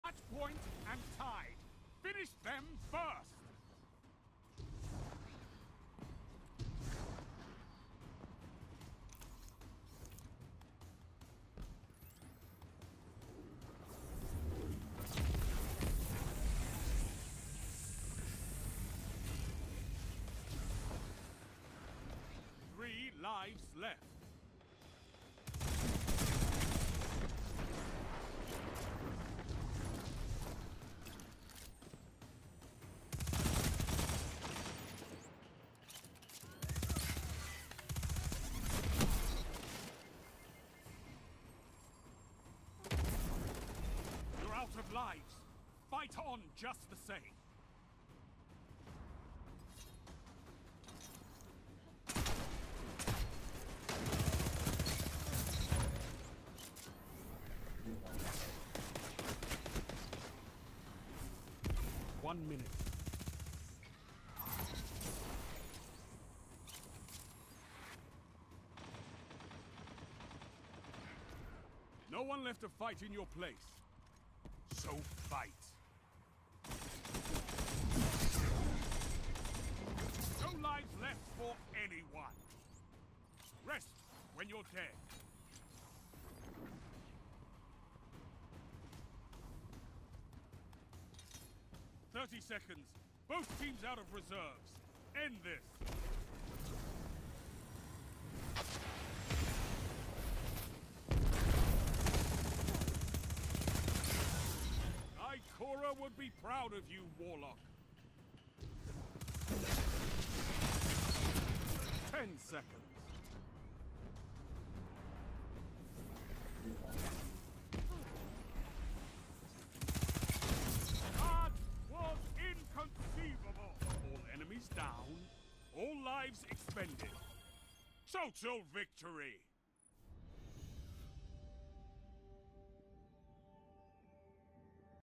5 Life Comeback, Final Round!
I was carrying a friend through Comp, but I didn't pay attention for the start of the game.

We brought it back from 0-3 to match point, but they weren't giving up on winning that easily...